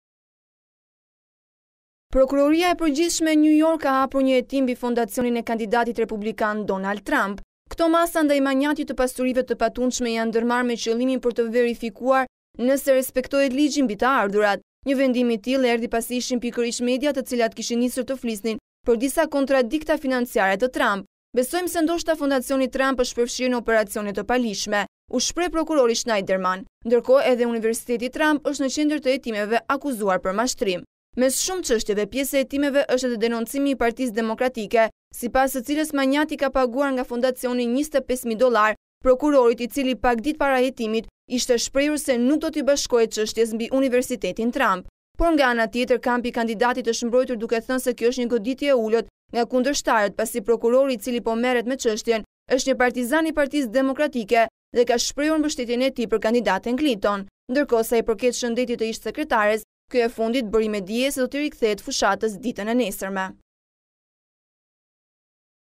Hetim fondacionit të Donald Trump dyshimi për donacione të paligjshme
Prokuroria e përgjithshme e New York ka hapur një hetim mbi fondacionin e kandidatit republikan Donald Trump.

Këto masa ndaj manjatit të pasurive të patundshme janë ndërmarrë me qëllimin për të verifikuar nëse respektohet ligji mbi të ardhurat.

Një vendim i tillë erdhi pasi ishin pikërisht mediat te vilat kishin nisur të flisnin për disa kontradikta financiare të Trump.

“Besojmë se ndoshta Fondacioni Trump është përfshirë në operacione të paligjshme” u shpreh prokurori Shnaiderman.

Ndërkohë edhe universiteti Trump është në qendër të hetimeve akuzuar për mashtrim.

Mes shumë çështjeve pjesë e hetimeve është edhe denoncimi i partisë demokratike sipas së cilës manjati i ka paguar nga fondacioni 25 mijë dollarë prokurorit i cili pak ditë para hetimit ishte shprehur se nuk do ti bashkohej cështjes mbi universitetin Trump.

Por nga ana tjetër kampi i kanidatit është mbrojtur duke thënë se kjo është një goditje e ulët nga kundërshtarët pasi prokurori i cili po merret me çështjen është një partizan i partisë demokratike dhe ka shprehur mbështetjen e tij për kandidaten Clinton.

Ndërkohë sa i përket shëndetit të ish sekretares kjo e fundit bëri me dije se do t’i rikthehet fushatës ditën e nesërme.